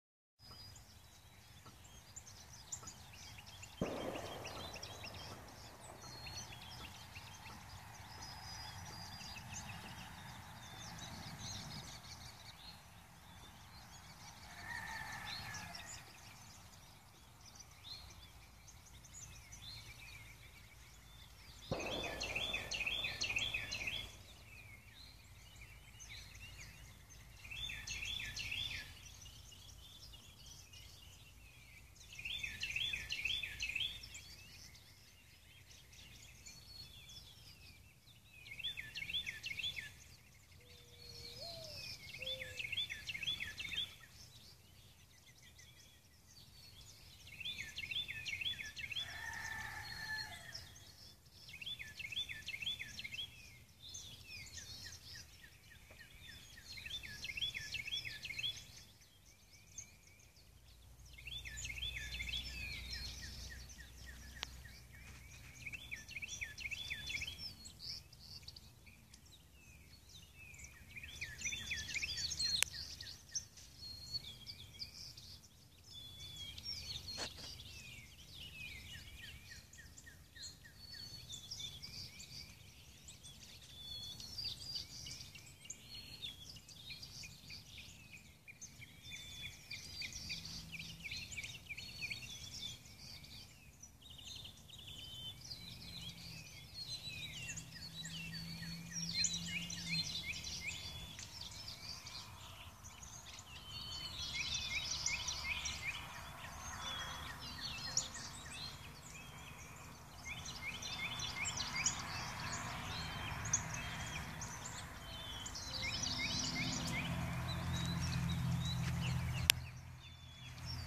Rabbits Playing Together 4-18-14
Make this 1080 and big screen to see. So I pulled the mower on up and the one who ran up the road had found his lady friend up there. They played and then a third came from the trees beside me.